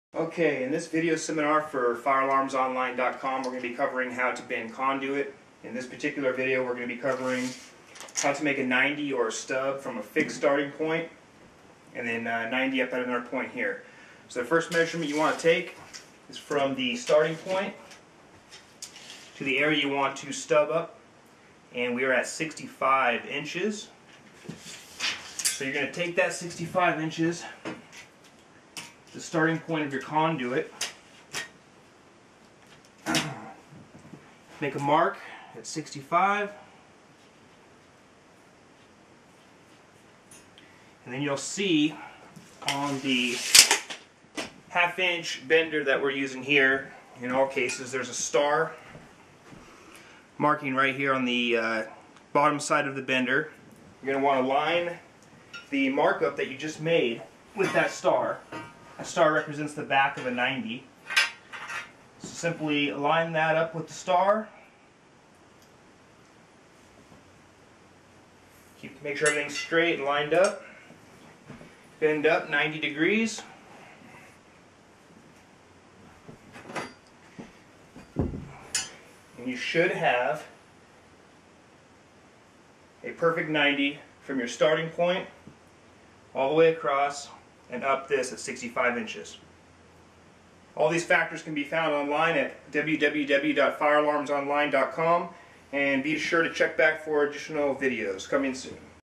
Conduit Bending How to Bend a Stub
A new version of this video can be found we will show you how to correctly bend a 90 degree or stub with a conduit bender.  Always use the star mark on the conduit bender to find the back of the 90 or bend.  Also be sure to join our Facebook group